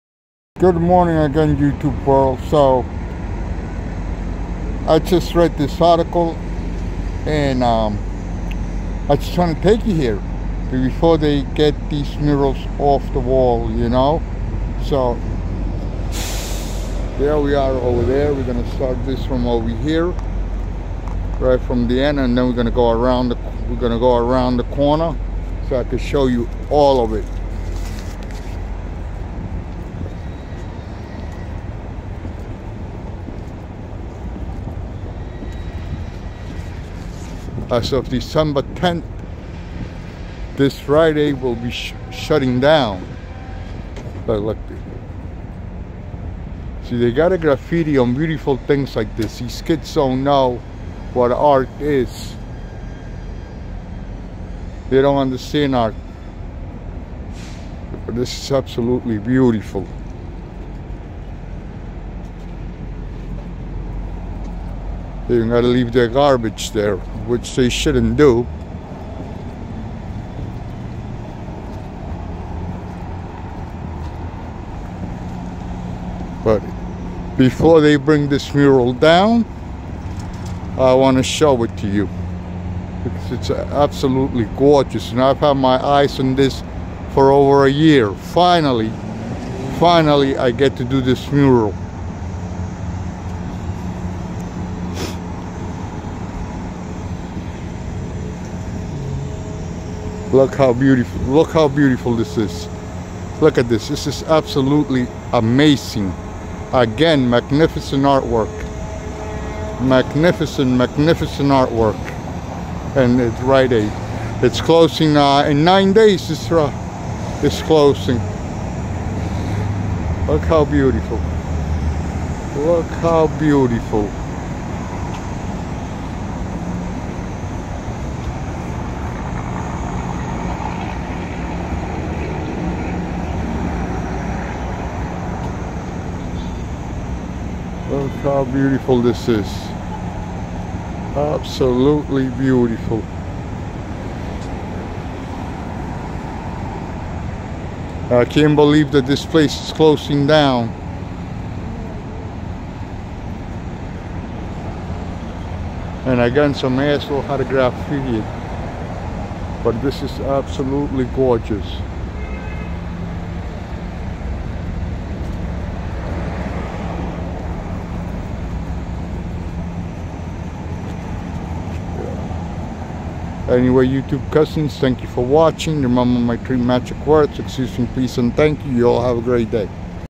Rite Aid December 1, 2023
Sunsetparkbrooklyn 4havenue and 62nd street.
This rite Aid is going to close December 10,2023 I wanted to bring this mural to my viewers before it goes away